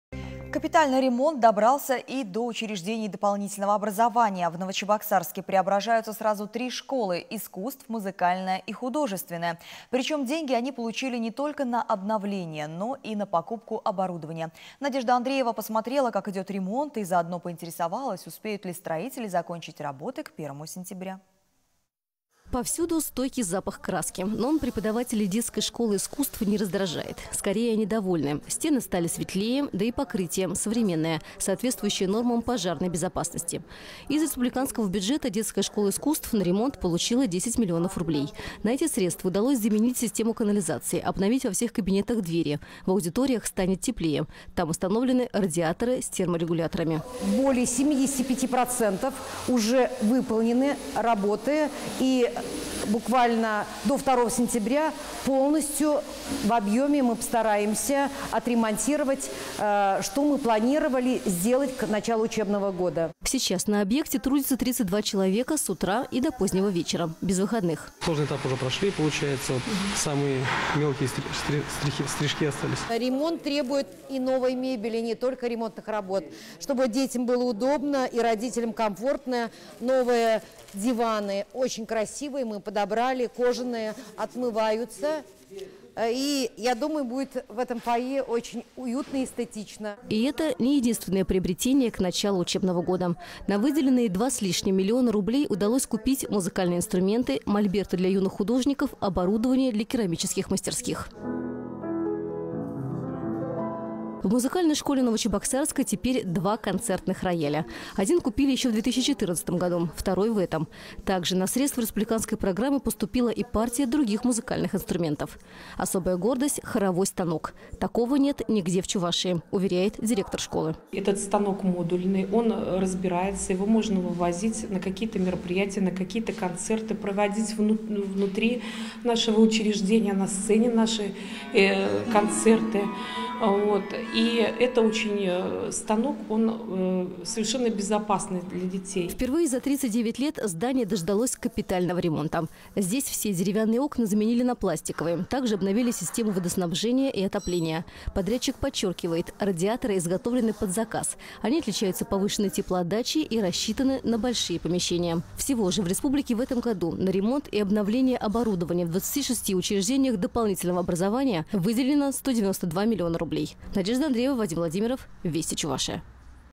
В Новочебоксарске преображаются и оснащаются новым оборудованием учреждения дополнительного образова
Капитальный ремонт добрался и до учреждений дополнительного образования. В Новочебоксарске преображаются сразу три школы - искусств, музыкальная и художественная.